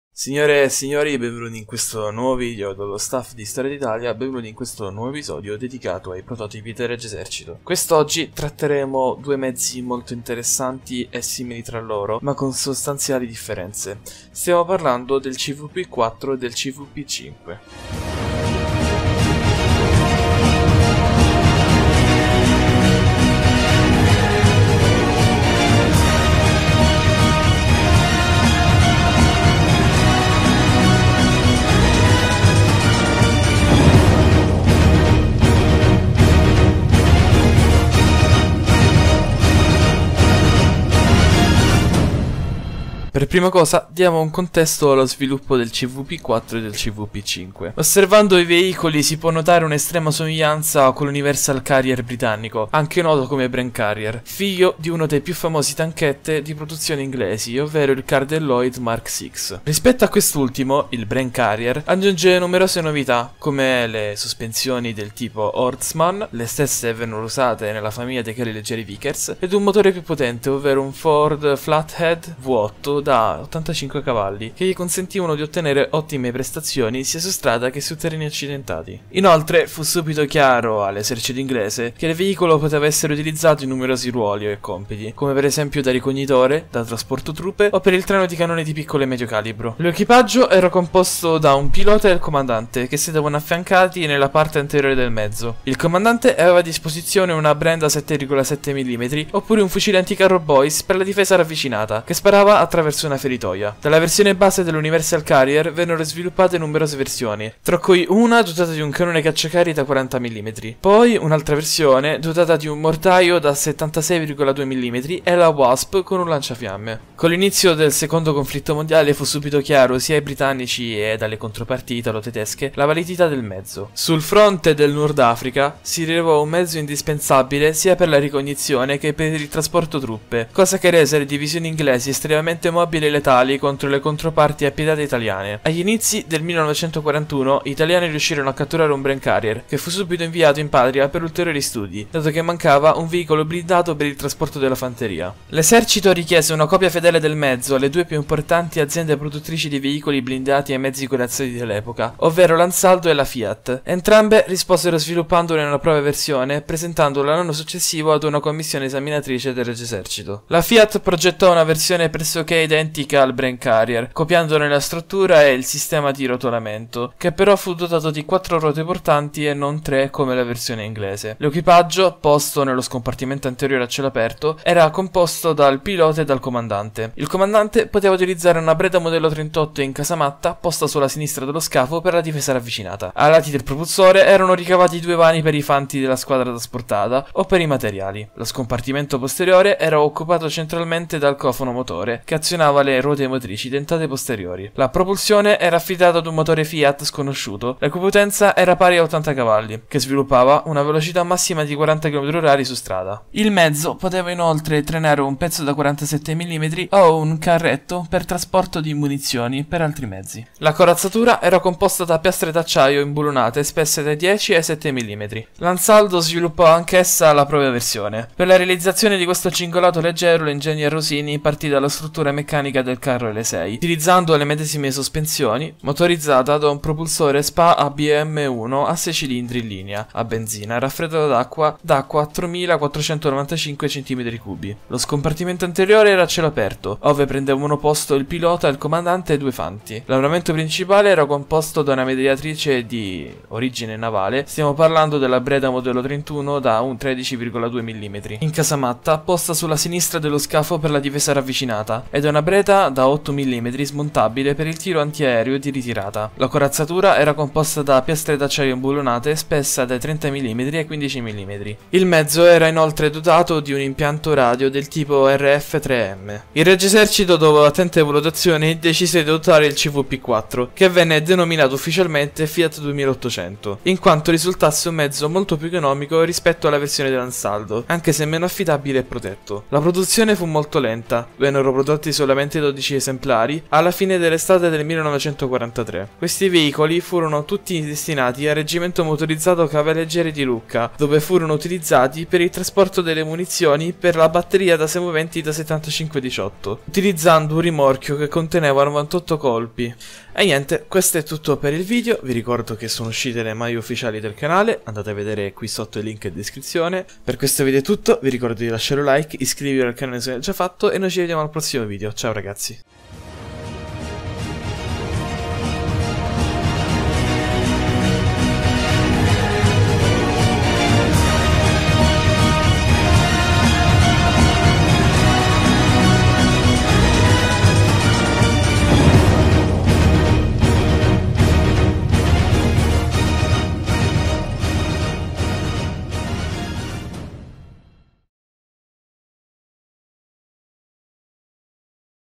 Fiat 2800 e ANSALDO L40 - Le copie italiana del UNIVERSAL CARRIER
!STORIA APOLITICA!

▬▬▬▬▬▬▬▬▬▬▬▬▬▬▬▬▬▬▬๑۩۩๑▬▬▬▬▬▬▬▬▬▬▬▬▬▬▬▬▬▬▬▬
Agli inizi del 1941 gli italiani riuscirono a catturare un Bren Carrier che fu subito inviato in patria per ulteriori studi dato mancava un veicolo blindato per il trasporto della fanteria. L’esercito richiese una copia fedele del mezzo alle due più importanti aziende produttrici di veicoli blindati e mezzi corazzati dell’epoca, ovvero la Ansaldo è la Fiat, entrambe risposero sviluppandone una propria versione presentandola l’anno successivo ad una commissione esaminatrice del Regio Esercito.
▬▬▬▬▬▬▬▬▬▬▬▬▬▬▬▬▬▬▬๑۩۩๑▬▬▬▬▬▬▬▬▬▬▬▬▬▬▬▬▬▬▬▬
Fonti:
Curami, L., Ceva, A. (1994). La Meccanizzazione dell’Esercito Italiano. Arte Della Stampa
Pignato, N, Cappellano, F. (2002). Gli Autoveicoli da Combattimento Dell’Esercito Italiano V.2. Stato Maggiore dell’Esercito